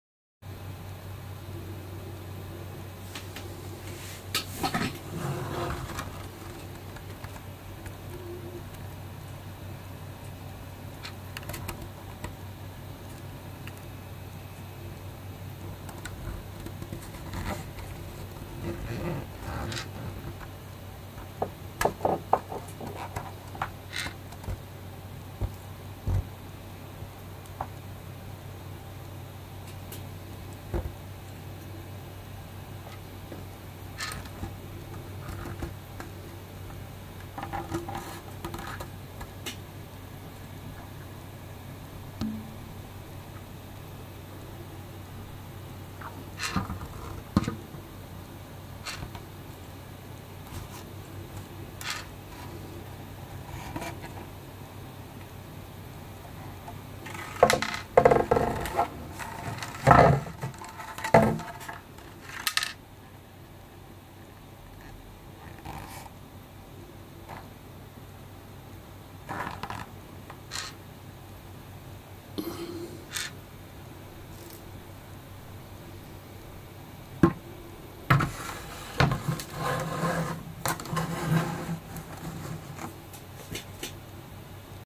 3D Printed Figure
Donkey Kong inspired Figure printed with a FDM 3D Printer.